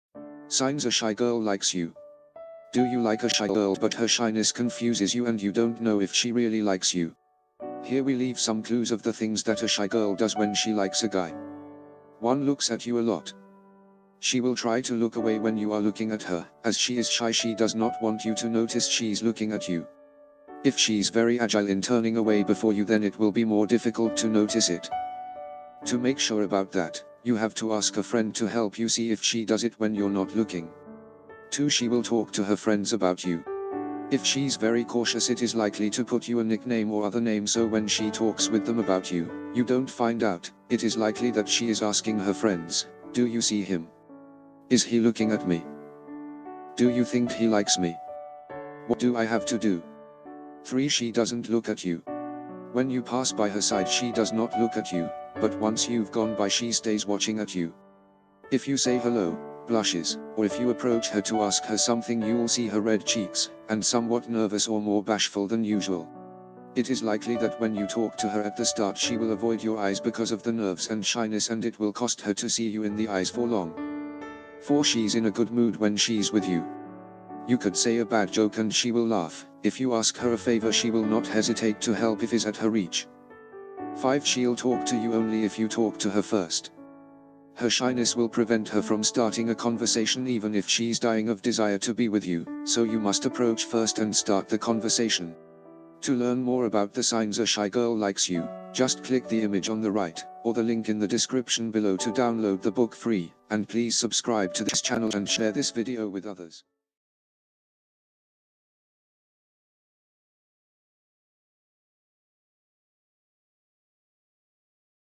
Signs A Shy Girl Likes You
Do you like a shy girl but her shyness confuses you and you don't know if she really likes you? Here we leave some clues of the things that a shy girl does when she likes a guy.

1. Looks at you a lot.

She will try to look away when you are looking at her, as she is shy she does not want you to notice she's looking at you. If she's very agile in turning away before you then it will be more difficult to notice it. To make sure about that, you have to ask a friend to help you see if she does it when you're not looking.

2. She will talk to her friends about you.

If she's very cautious it is likely to put you a nickname or other name so when she talks with them about you, you don't find out, it is likely that she is asking her friends: Do you see him? Is he looking at me? Do you think he likes me? What do I have to do?

3. She doesn't look at you.

When you pass by her side she does not look at you, but once you've gone by she stays watching at you.

If you say hello, blushes, or if you approach her to ask her something you'll see her red cheeks, and somewhat nervous or more bashful than usual.

It is likely that when you talk to her at the start she will avoid your eyes because of the nerves and shyness and it will cost her to see you in the eyes for long.

4. She's in a good mood when she's with you.

You could say a bad joke and she will laugh, if you ask her a favor she will not hesitate to help if is at her reach.

5. She'll talk to you only if you talk to her first.

Her shyness will prevent her from starting a conversation even if she's dying of desire to be with you, so you must approach first and start the conversation.